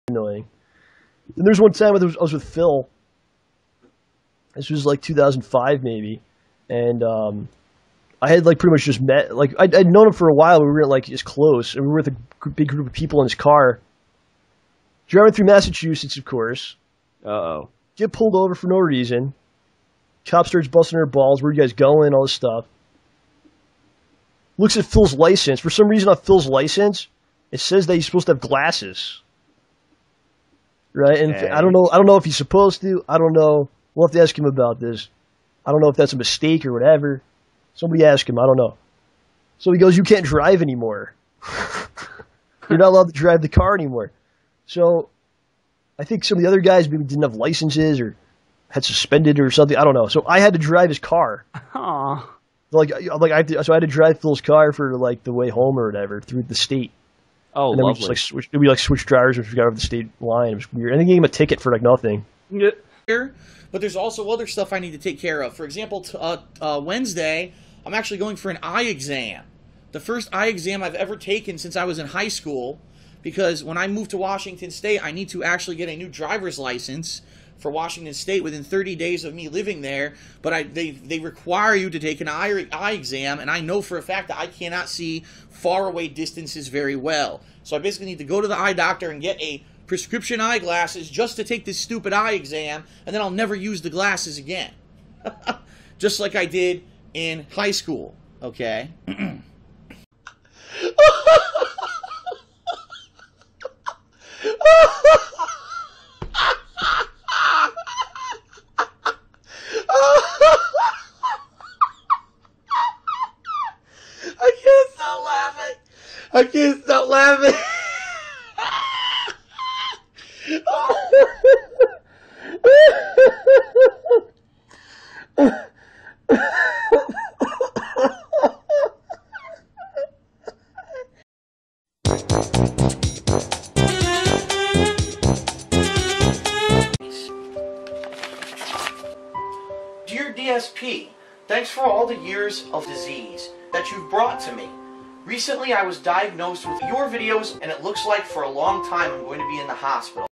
DSP Tries It: Look No Glasses.
Well well mr.phil burnell(aka dspgaming), it turns out you lied to your own best friend John Rambo and your friends in a car that you did not need glasses and put them in danger, along with everyone else on the road. Now prepare to be exposed for it, especially when you called the fans of Dark Souls, and Demon Souls that tried to critique you for abusing cheats to beat the game that they wear glasses and they are "perfectionist assholes".

Sources:

Images

The tweet that started it all

Videos

DSP and His Bugged, Bat-like Vision

DSP Is Blind

Music

Sounds

DSP lolgasm.